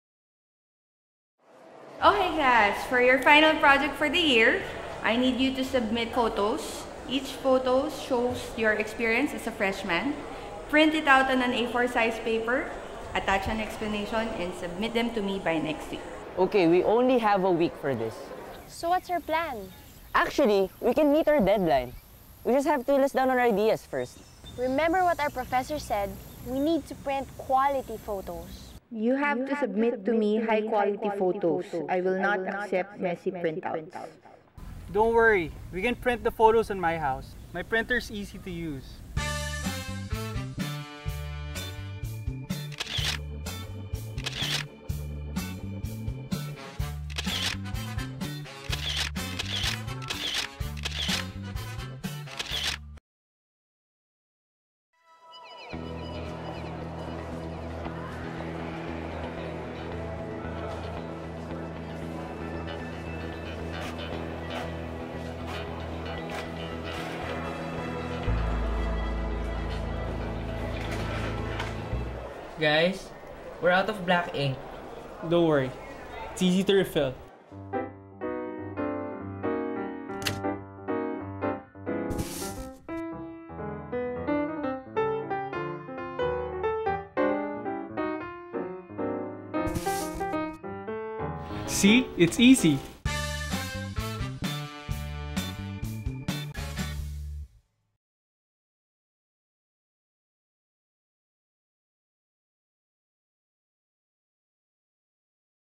Epson L-Series - Ease Of Use: School (Philippines)
Students who want high-quality prints to meet their rigorous school requirements choose Epson's L-Series genuine ink tank system printers. By offering the lowest printing costs against any branded printer in the market that uses genuine consumables, the L-Series is the smart choice for the discerning student.